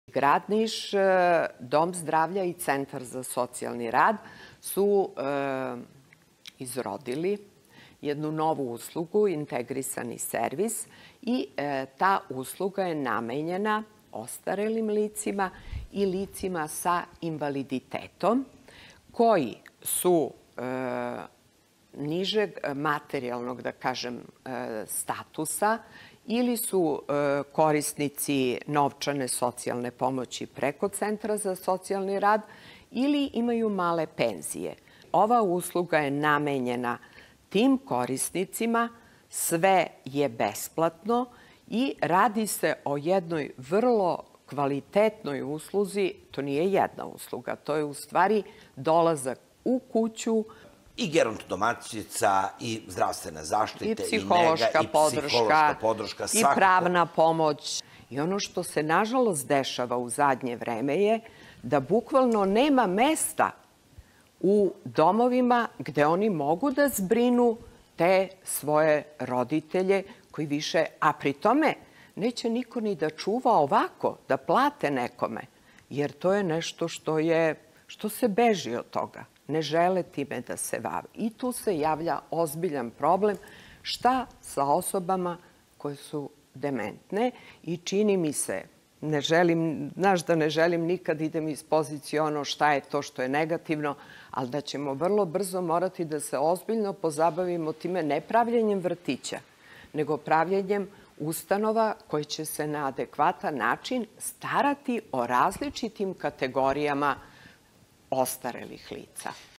U SUSRET STARIMA – CENTAR INTEGRISANIH USLUGA
Iz godine u godinu broj staračkih samačkih domaćinstava raste, to pokazuju i rezultati poslednjeg popisa stanopvništva. Sve je veći broj dementnih i bolesnih. Nameće se velika potreba otvaranja novih domova za stare, ali i pružanja raznih oblika pomoći u njihovim domovima. Nova usluga u Nišu namenjana je za pomoć upravo osobama iz tih kategorija.